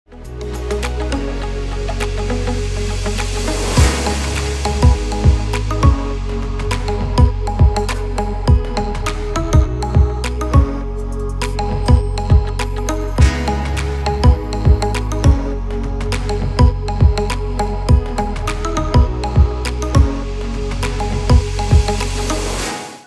3 Looks, 1 Palette!
let your aura sparkle with our All Amethyst palette ft purple, lilac, and duochrome shades! 🦄